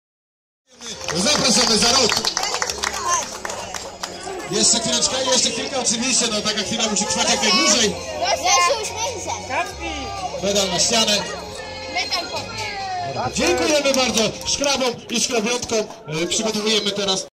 MTB Jesień w Mieście Cudów - 27 września 2015 roku Bardo cz. 15
Grupowe zdjęcie najmłodszych MTB Jesień w Mieście Cudów - 27 września 2015 roku Bardo cz. 15